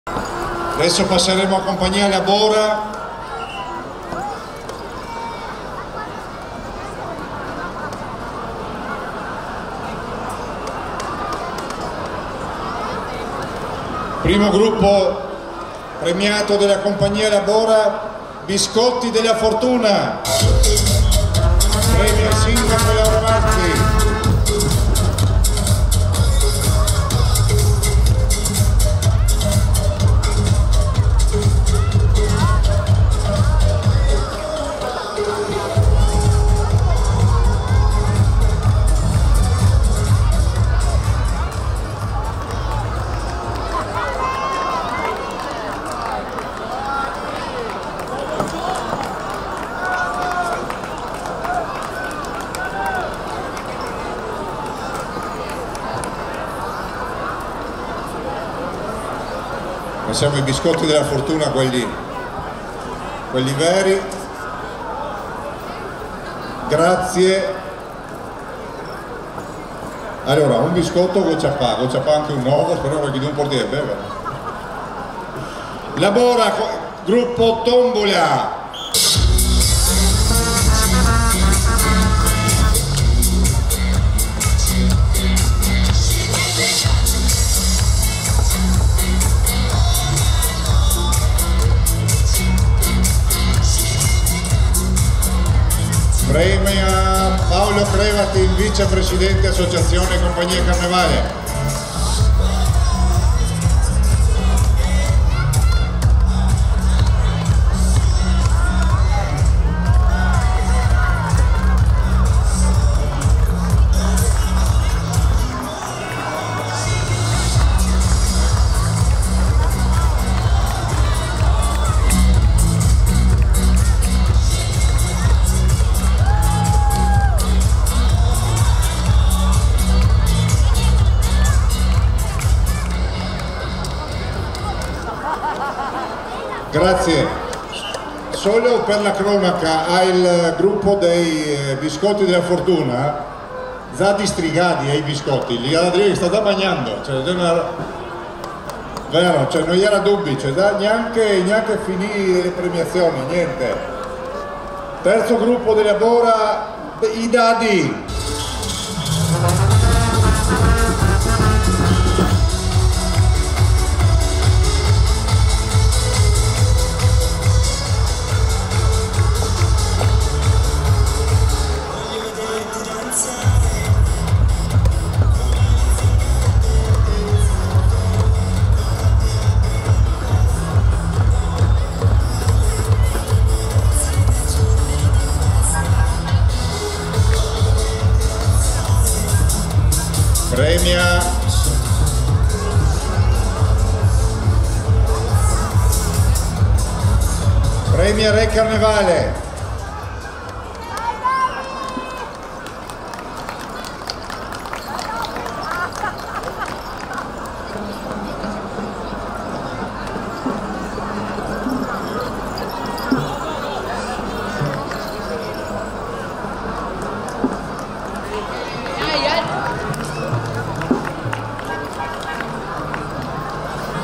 Premiazione maschere La Bora
66° carnevale muggesano
05/03/2019
PREMIAZIONE MASCHERE CHE SI SONO DISTINTE:
• BISCOTTI DELLA FORTUNA
• TOMBOLA
• DAD